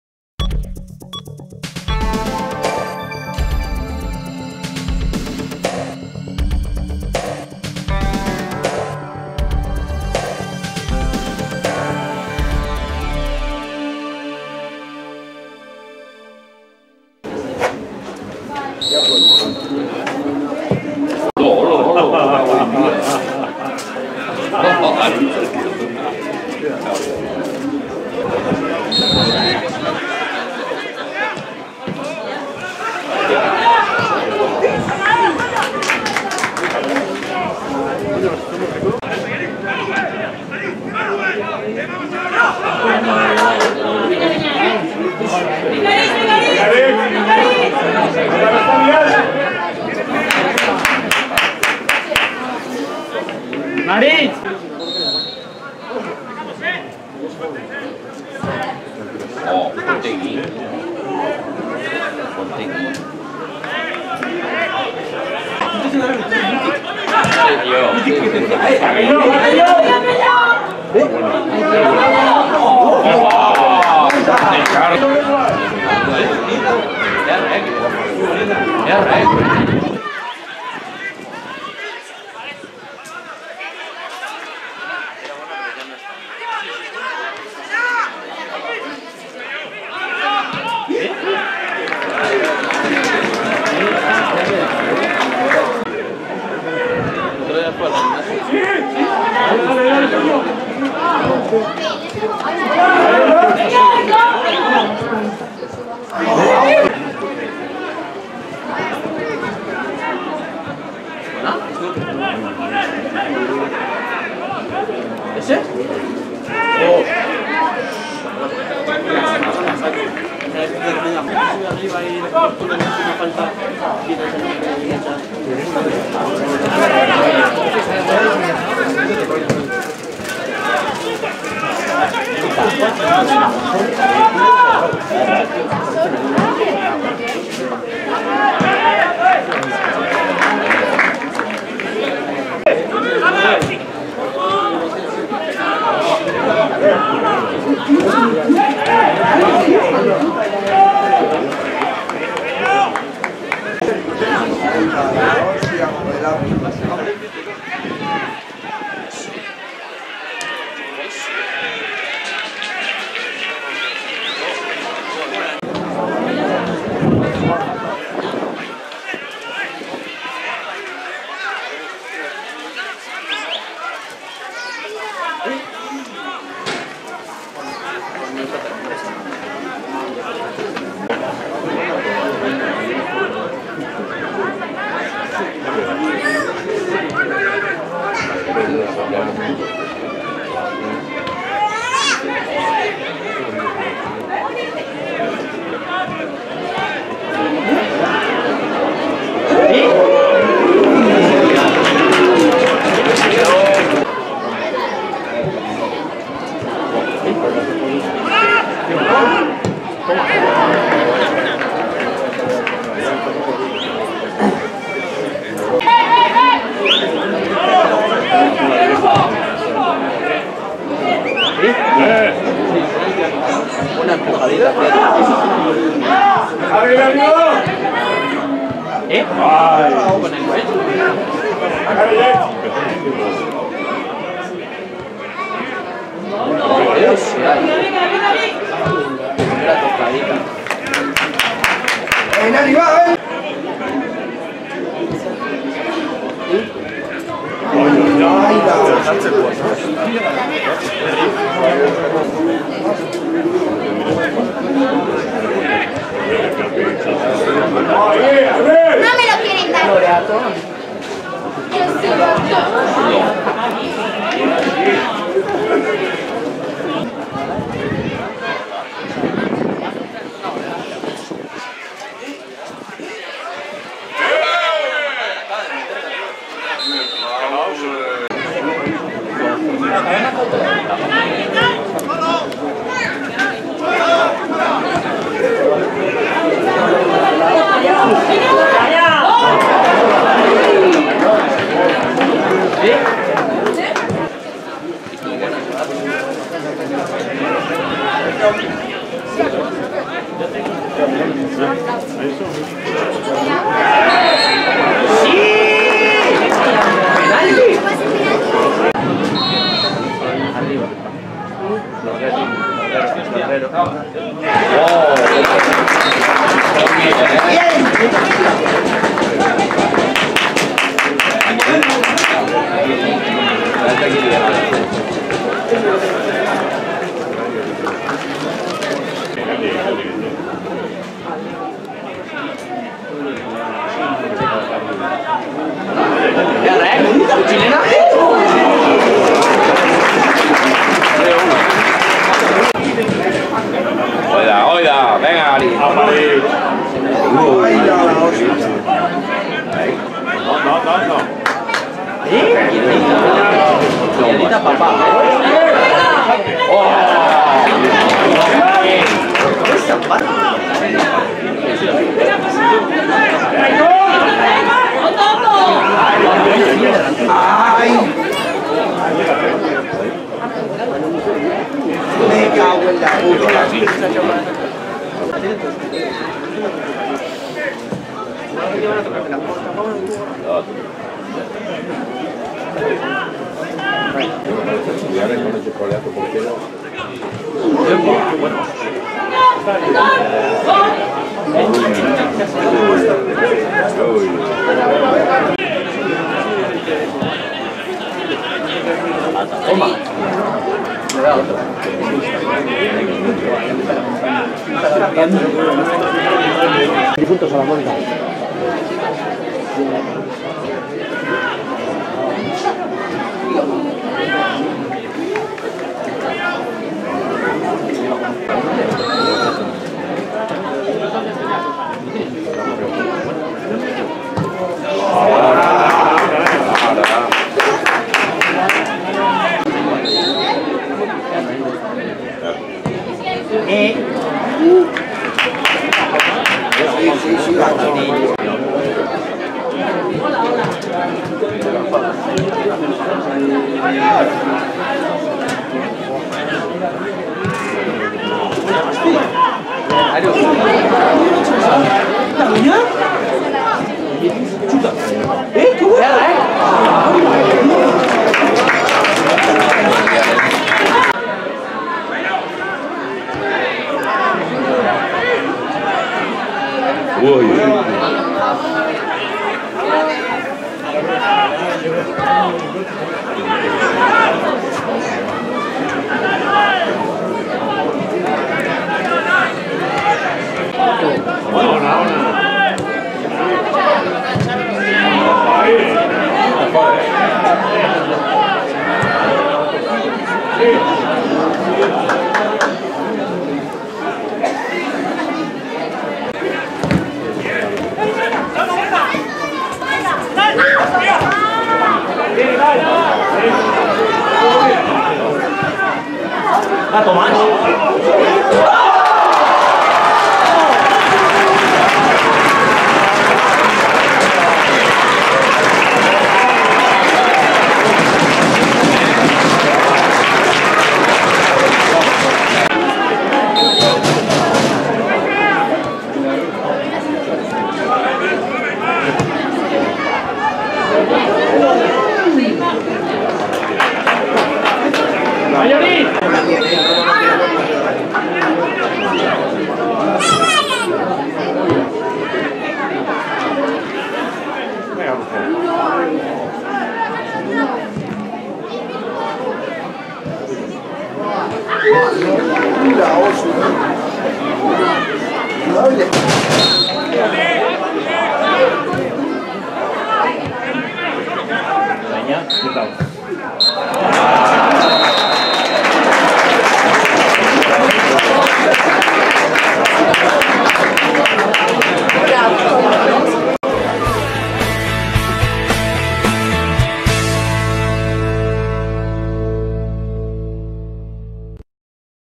AMAIKAK-BAT F.T. 1:1 C.D. Hernani (Errotazar, 2023/12/03)
2023ko Abenduaren 3an Debako Errotazar futbol zelaian jokatu zen partiduaren laburpena.